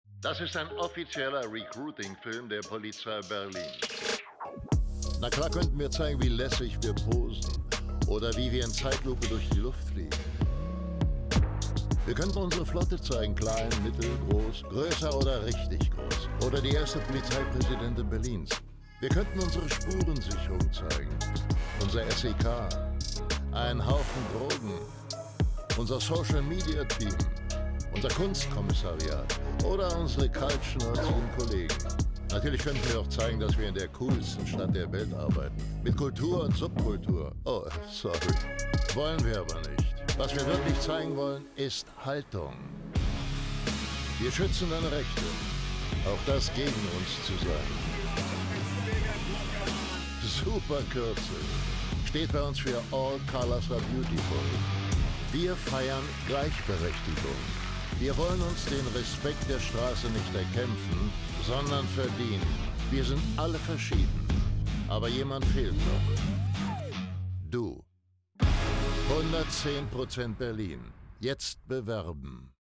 110% Berlin: Das ist ein offizieller Recruitingfilm der Polizei Berlin (aber nicht, was ihr denkt)
Die Polizei Berlin sucht neue Mitarbeitende. In Uniform oder in Zivil – bei der Hundertschaft und der Hundestaffel, im Drogen- und im Kunstdezernat, beim SEK und LKA, bei der Fahrrad- und der Motorradstaffel, bei der Spurensicherung und dem Social-Media-Team, in den Direktionen und im Kiez, mit MSA oder Abitur, als Athlet oder als Ärztin, Physikerin oder Psychologe, als Kaufmann oder Kraftfahrzeugmechatronikerin. Wir stehen auf Diversität. Und auf Bewerbungen von Frauen, Männern und allen anderen Geschlechtern aus Europa, wenn du zwischen 16 und 39 Jahren bist.

Wenn du den Film doof findest, aber die Polizei gut, bewirb dich

Wenn du uns total doof findest: Wir schützen dich trotzdem.

Dieser Kurzfilm entstand in Kooperation mit glow communication GmbH.